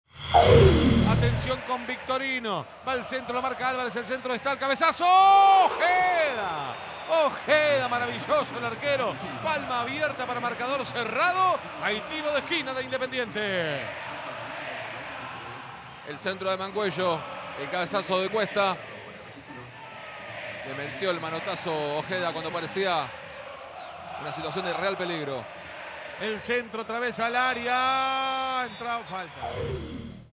Buena reacción de Ojeda. Independiente 0 - Argentinos 0. Fecha 10. Primera División 2015. FPT.
Mirá la atajada del arquero del Bicho para mantener su arco en cero.